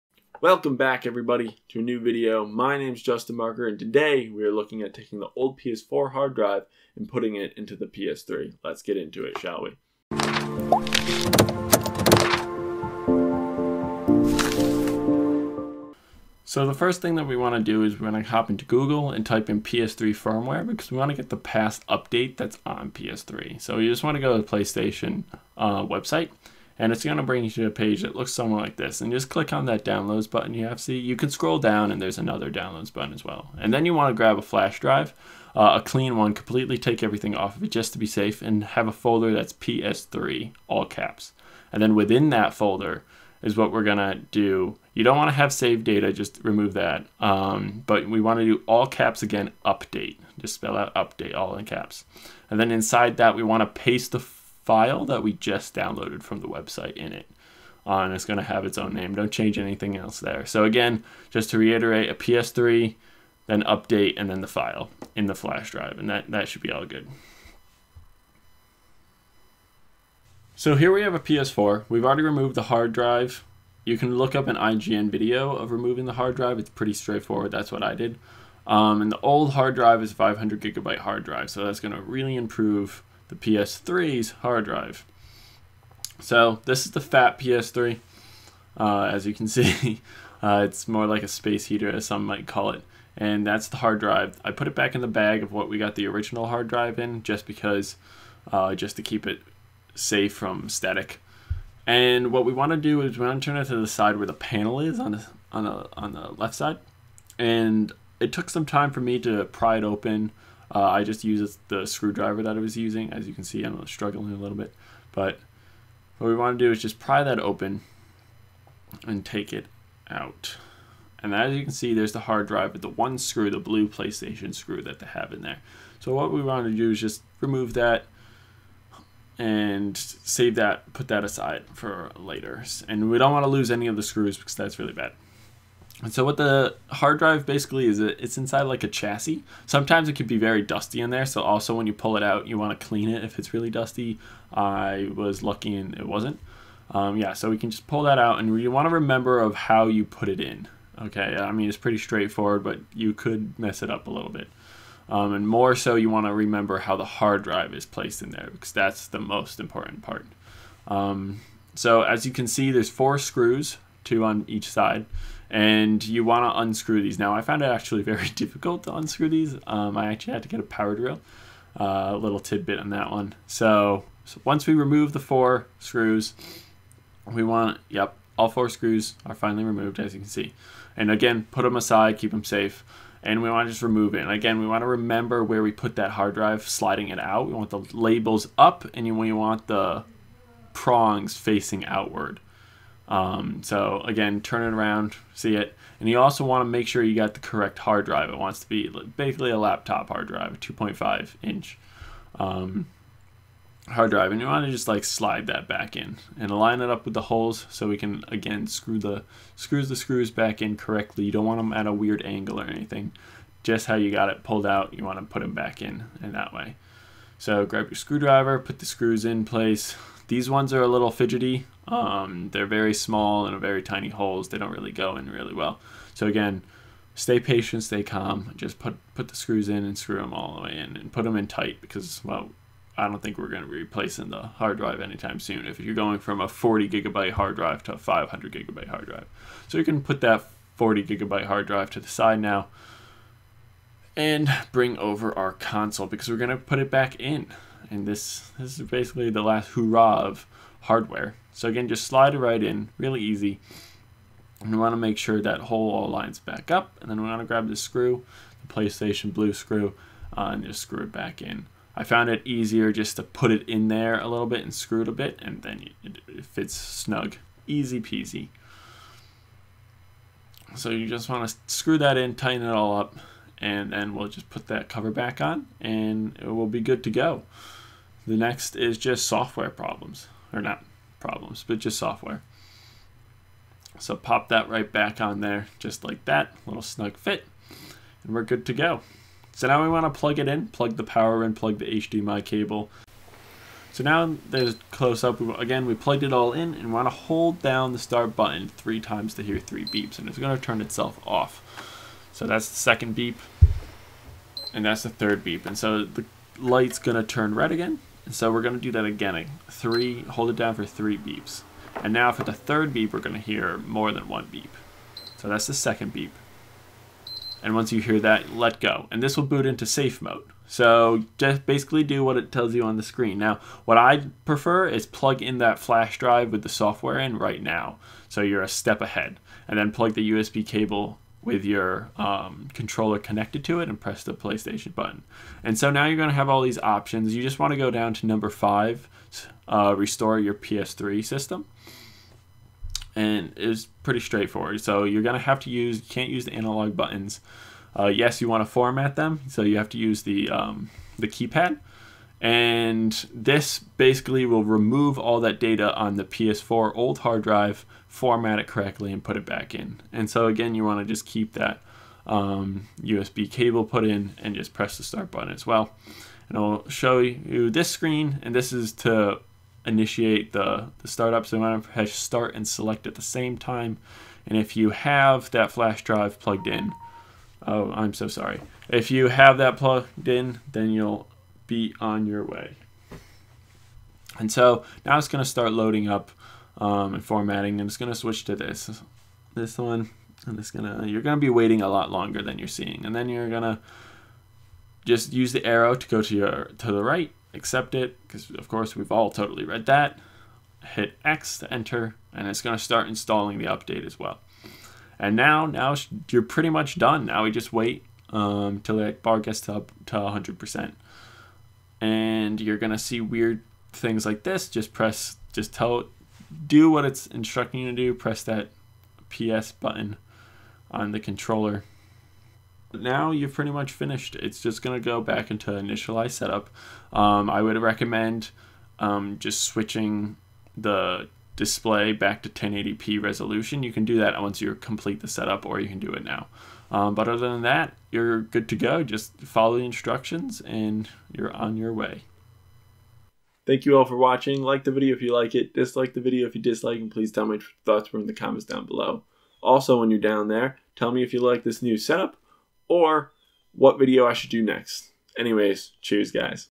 How to Upgrade your PS3's Hard Drive using your old PS4's Hard Drive
If you already upgraded your PS4's Hard Drive with something a little bit bigger. You probably have that 500Gb hard drive just sitting around. Let's put it to good use, shall we?

I show you how to switch out the PS3's hard drive with the old PS4's 500GB hard drive. It'll give you a little more than 12x the space. Allowing you to play as many games as you desire.

If you do not follow all the step correctly, or if something goes wrong I am not liable for any damages to your devices.

If you want to upgrade your PS3 or PS4's hard drive here are some options:

2Tb Hard Drive

1Tb Hard Drive

500Gb Hard Drive